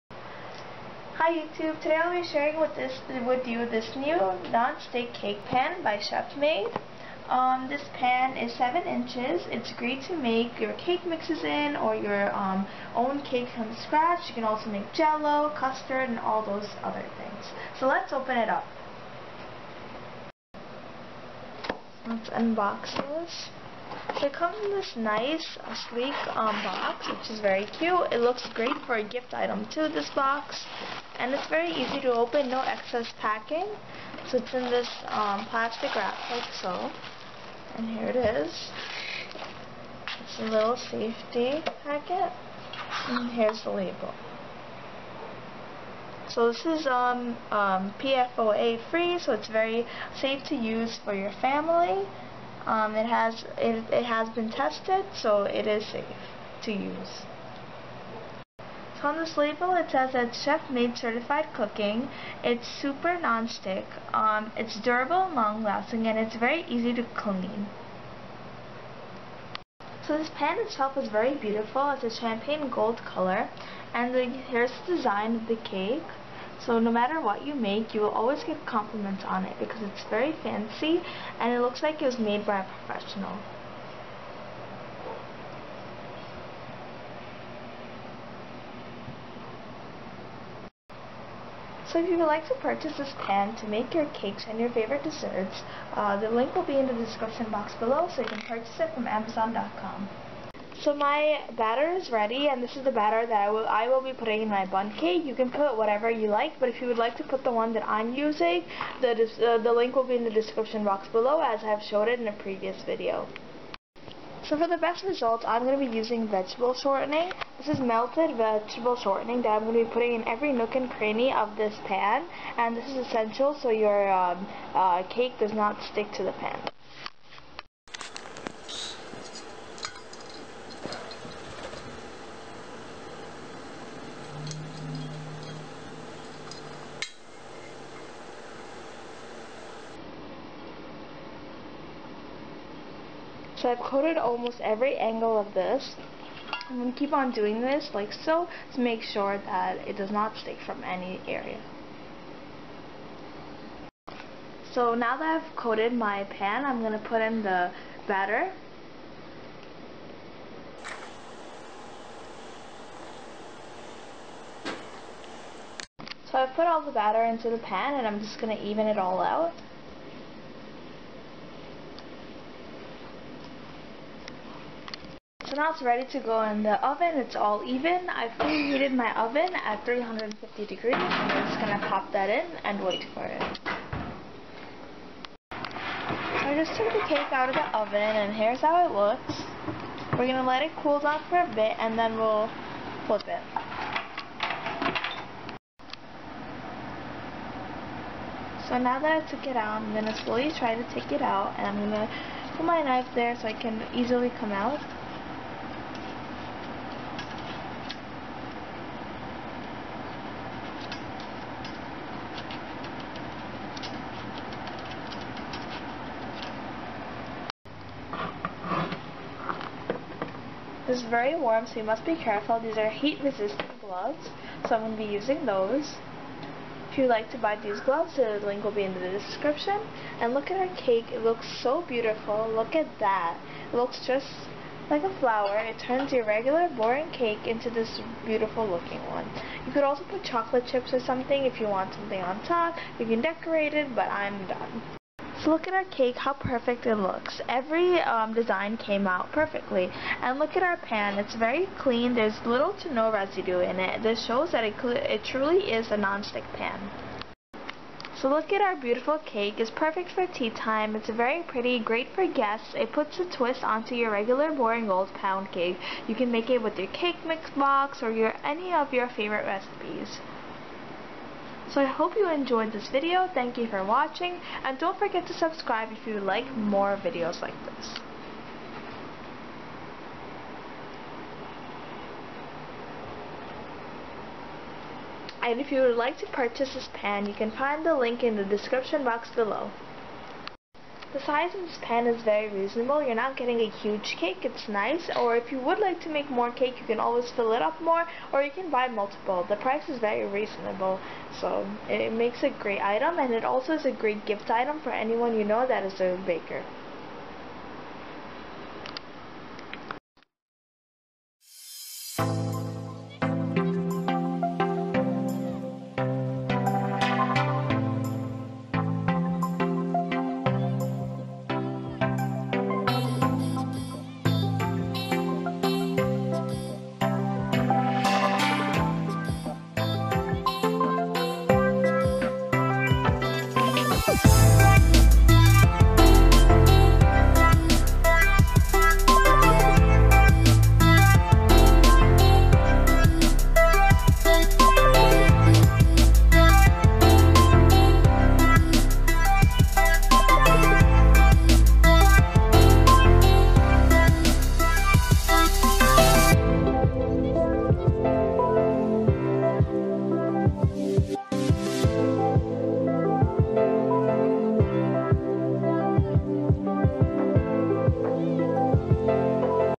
Bundt Cake - Easy Tea Time Cake - Pakistani/Indian Cooking with Atiya
How to make a beautiful delicious tea time bundt cake in a non-stick bundt cake pan by chefmade.
CHEFMADE Bundt Cake Pan, 7-Inch Non-Stick Vortex-Shaped Kugelhopf Bakeware, FDA Approved for Oven and Instant Pot Baking (Champagne Gold)
To purchase pan
To purchase gloves